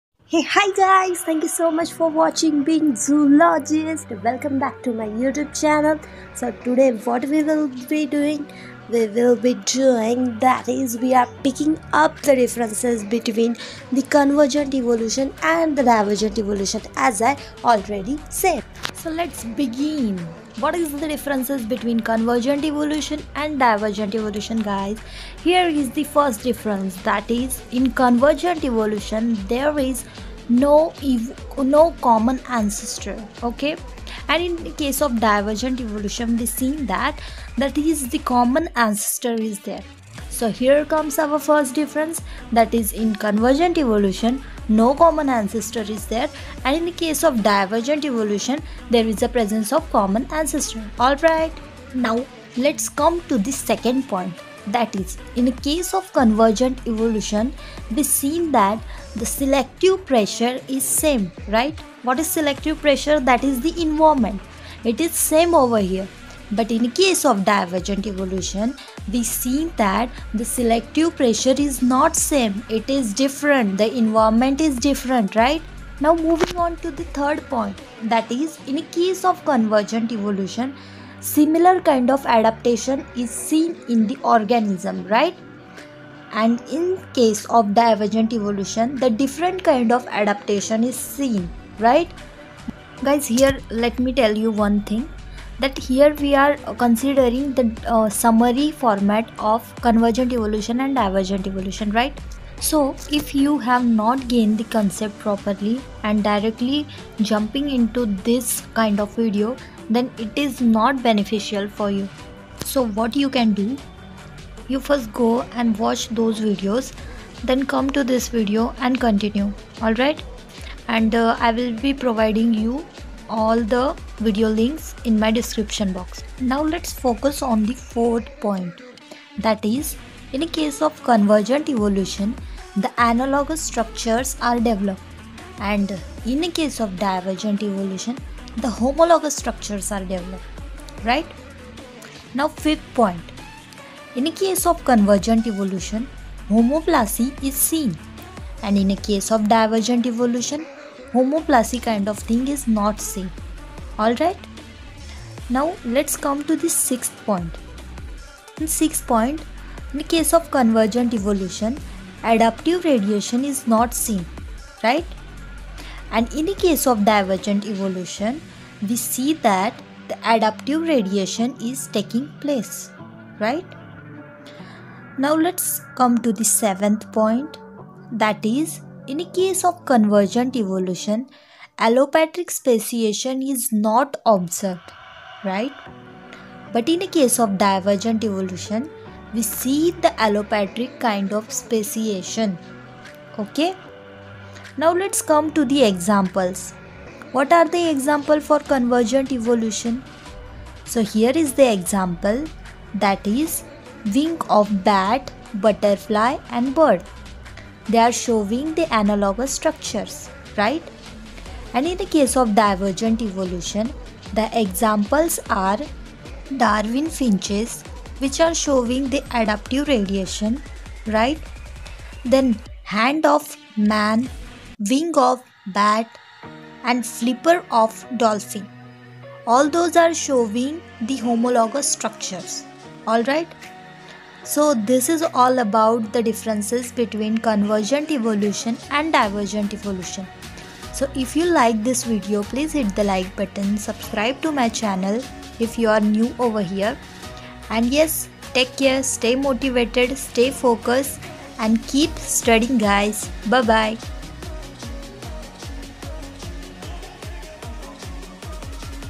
Difference between convergent evolution and Divergent evolution
1.basic difference between convergent and divergent evolution with example is explained
2.helpful for quick revision
     This video will help you to distinguish between convergent evolution and divergent evolution... I have added all the important points which are important for your exam point of view. After watching this video I don't think you feel any kind of difficulties to remember both the concepts. although you can go and watch the detail explanation of both topics separately for that check out the links below

until we meet
stay focused
stay motivated
and keep learning.